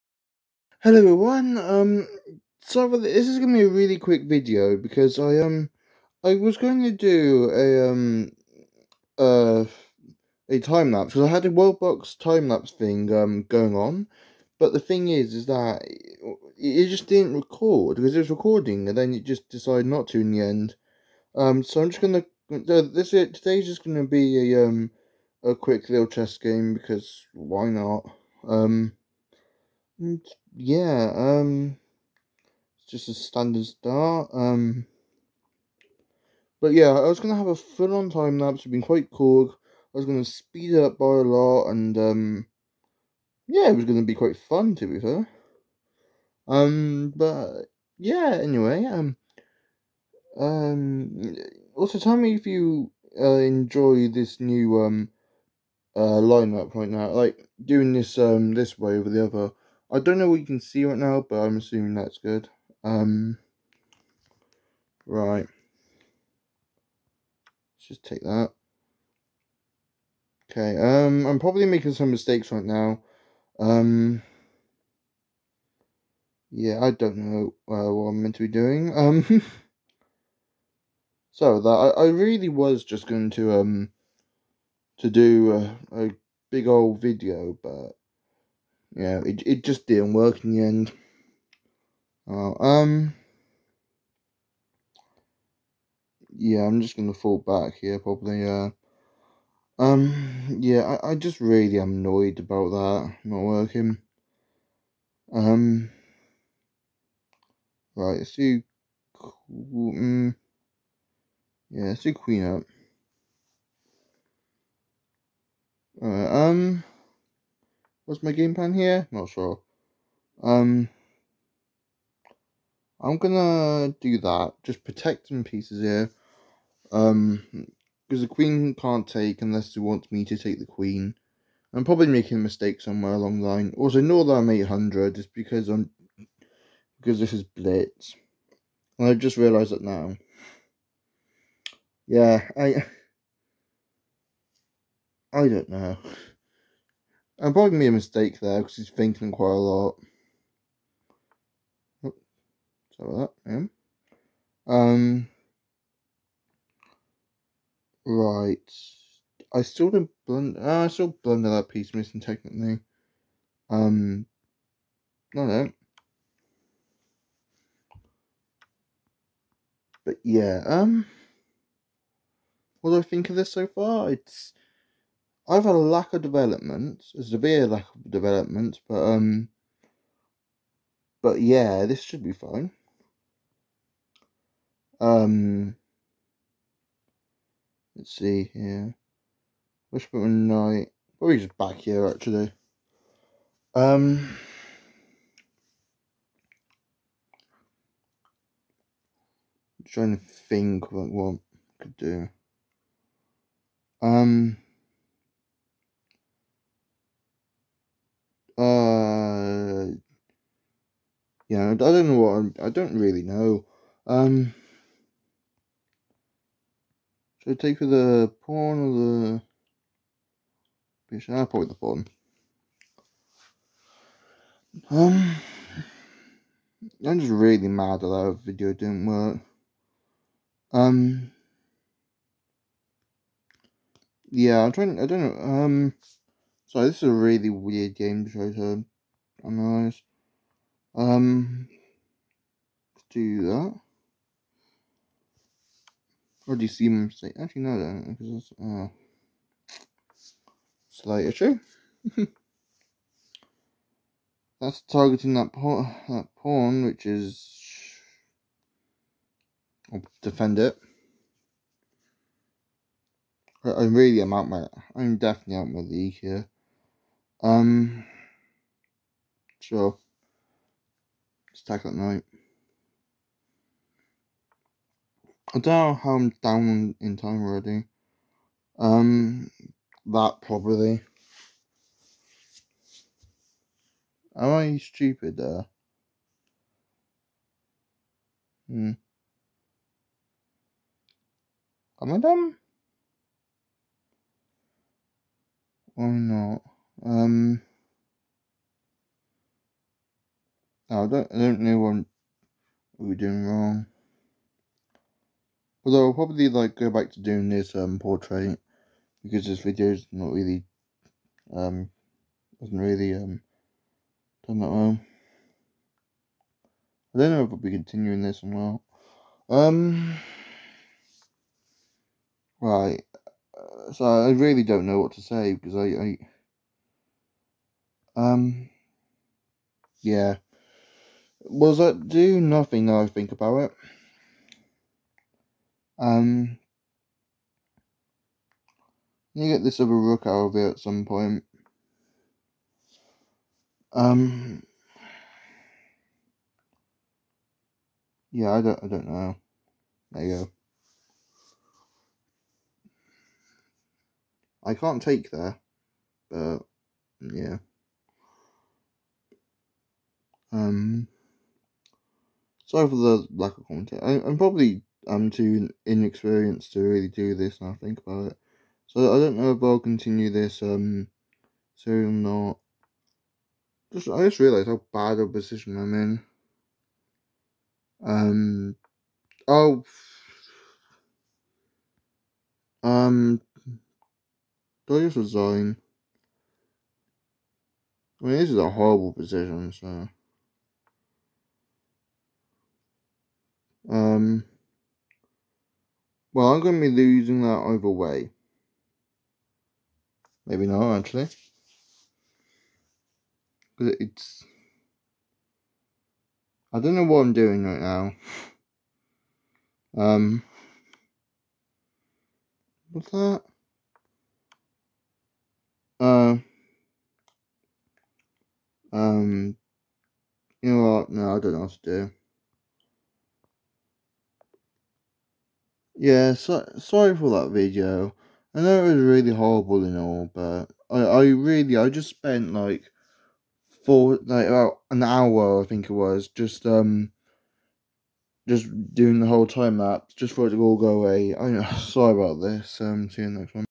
Getting DESTROYED while ranting about a ruined recording
It still annoys me now

the_learning_gamer1